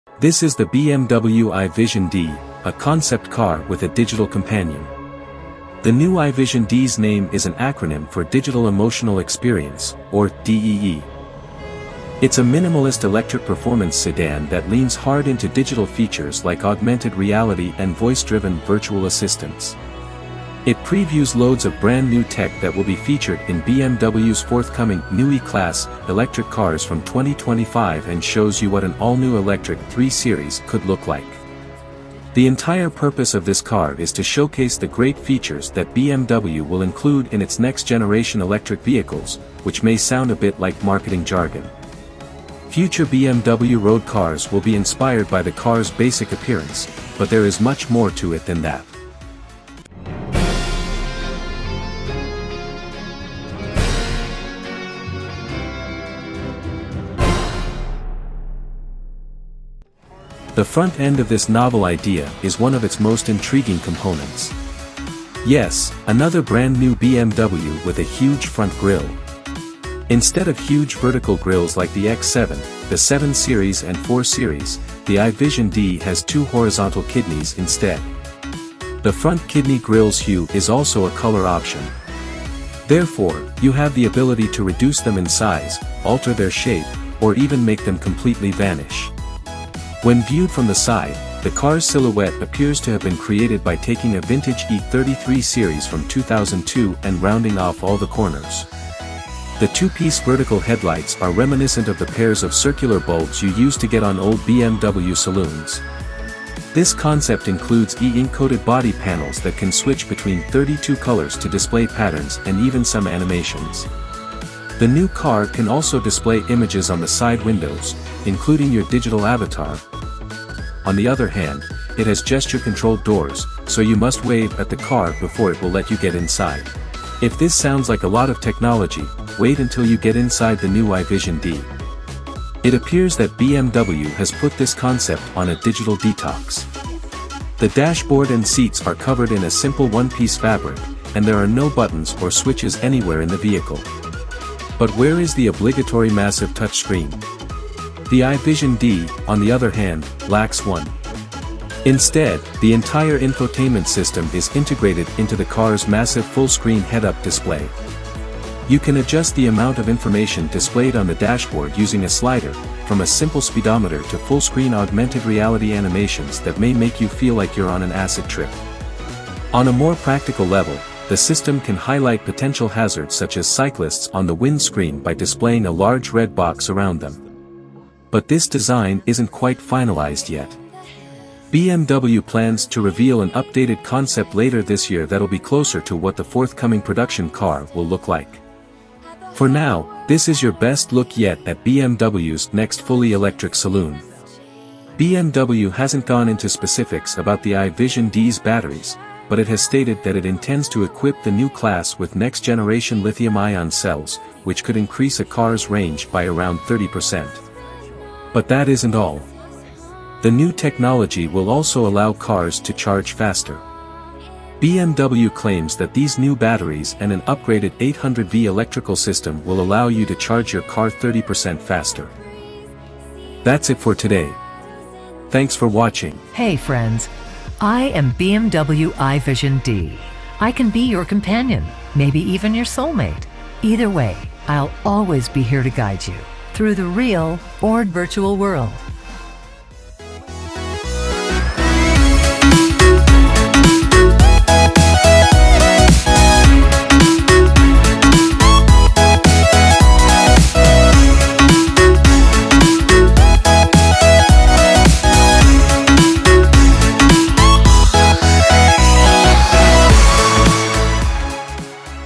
World's First Color Changing Car With a Digital Soul
This is the new BM i Vision Dee concept, the world's first car with a digital soul.

Imagine technology infused with a human touch. A machine whose intelligence is emotional rather than artificial. A vehicle with a voice, facial expressions, and true personality. A car that’s more than a means of transportation, but an ultimate companion.

But, there is more to that, the i Vision Dee will also come with E-ink technology, allowing you to change the car's exterior colour!